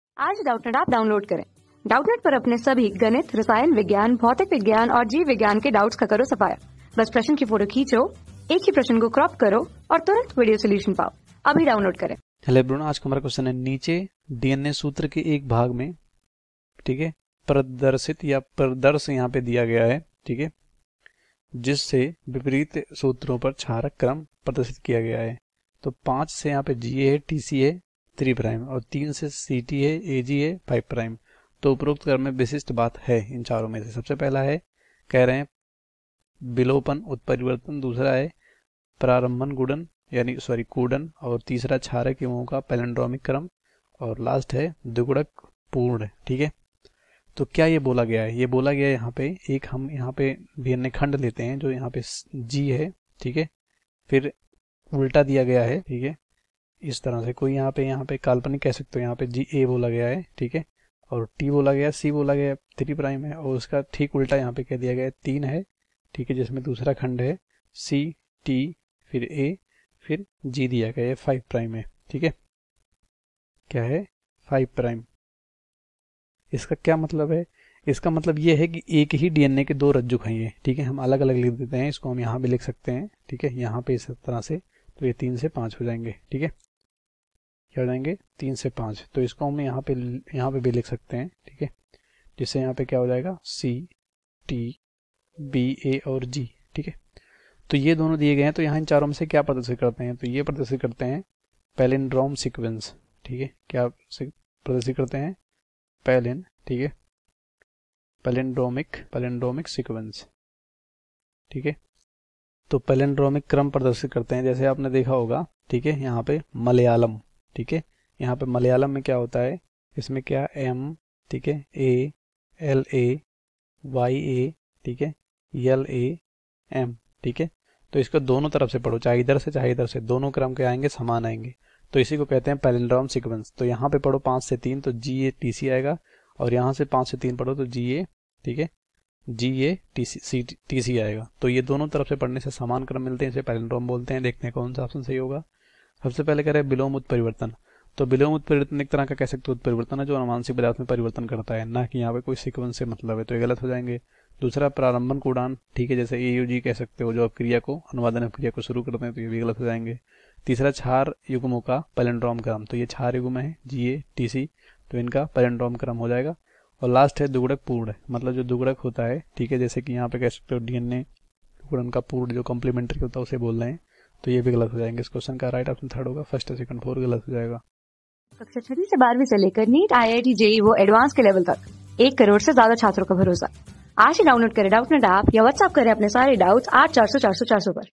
निचे DNA सूत्र के एक भाग में प्रदिदर्श दिया गया है, जिसमे विपरीत सूत्रों पर क्षारक क्रम प्रदर्शि...
निचे DNA सूत्र के एक भाग में प्रदिदर्श दिया गया है, जिसमे विपरीत सूत्रों पर क्षारक क्रम प्रदर्शित किया गया है। 5\'-GAATTC-3\' 3\'-CTTAAG-5\' उपरोक्त क्रम में विसिष्ट बात है
Class: 12
Subject: BIOLOGY
Chapter: जैव-प्रौद्योगिकी : सिद्धान्त एव अनुप्रयोग
Board:NEET

👉 Have Any Query? Ask Us.